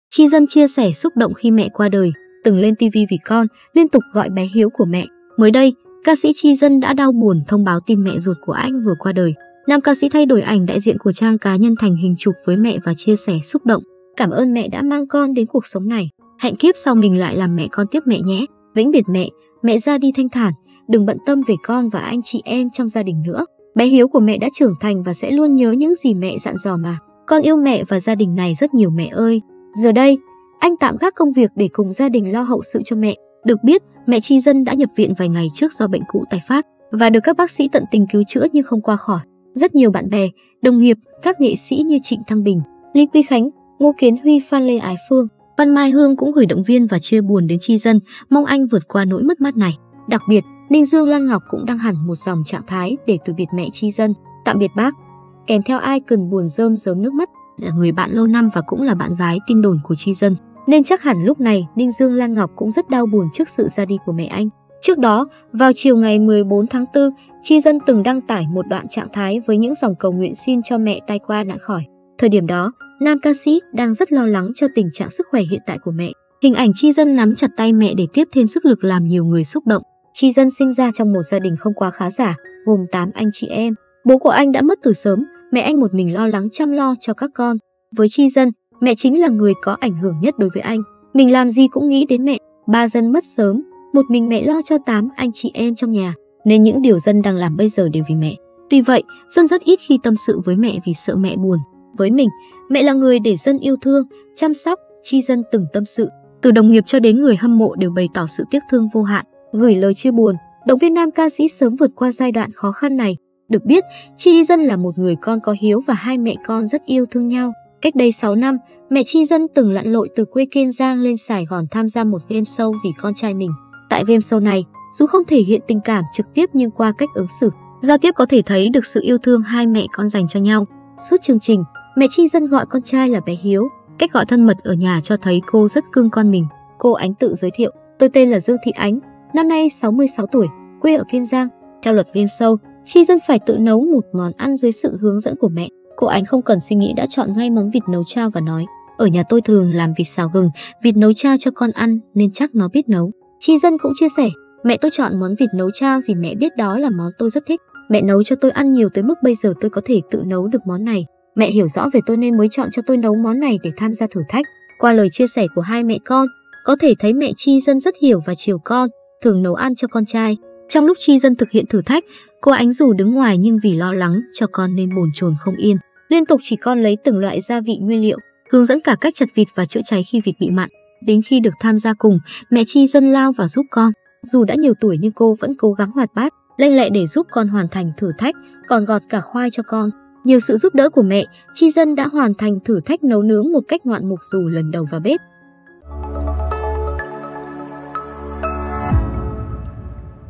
Chi Dân chia sẻ xúc động khi mẹ qua đời, từng lên tivi vì con, liên tục gọi “bé Hiếu” của mẹ
Mới đây, ca sĩ Chi Dân đã đau buồn thông báo tin mẹ ruột của anh vừa qua đời. Nam ca sĩ thay đổi ảnh đại diện của trang cá nhân thành hình chụp với mẹ và chia sẻ xúc động: "Cảm ơn mẹ đã mang con đến cuộc sống này, hẹn kiếp sau mình lại làm mẹ con tiếp mẹ nhé, vĩnh biệt mẹ, mẹ ra đi thanh thản, đừng bận tâm về con và anh chị em trong gia đình nữa, bé Hiếu của mẹ đã trưởng thành và sẽ luôn nhớ những gì mẹ dặn dò mà, con yêu mẹ và gia đình này rất nhiều mẹ ơi".